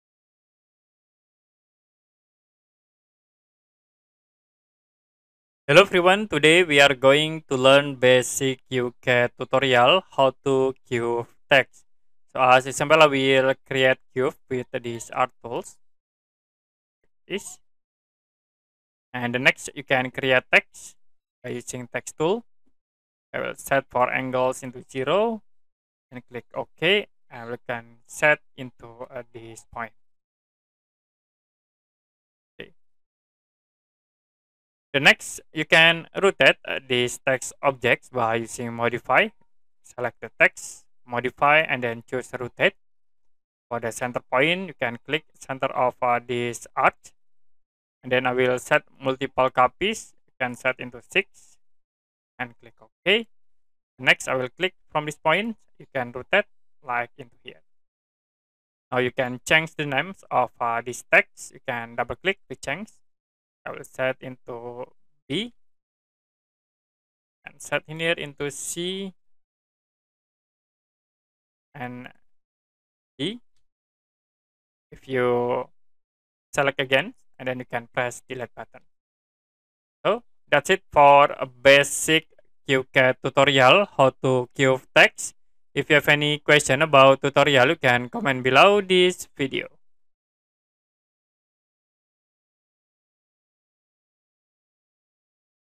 How To Curve Text In QCAD
Learn how to curve text in QCAD with our easy step-by-step guide. Master this essential skill to enhance your designs and create professional-looking projects effortlessly.